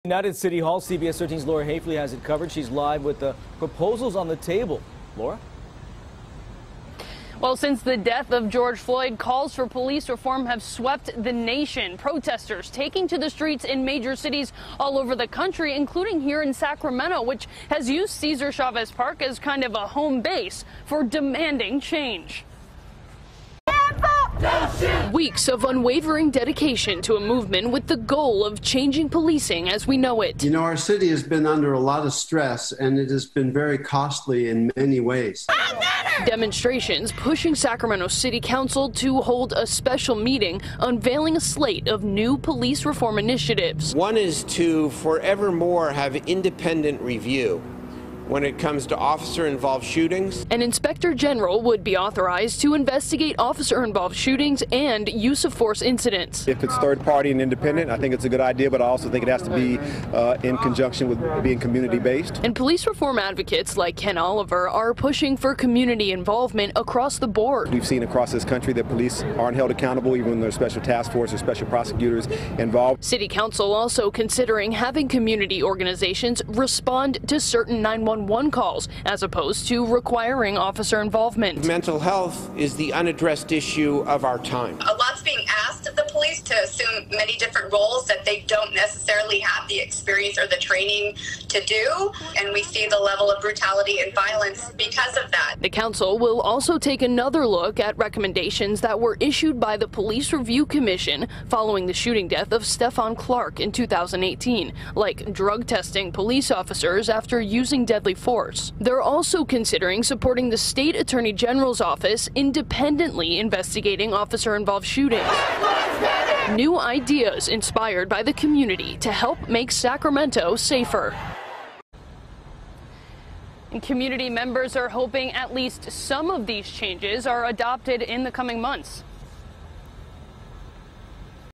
Sacramento Leaders Discuss Police Reform Plans
The Sacramento City Council held their much-anticipated meeting surrounding possible police reform in the City of Sacramento Wednesday.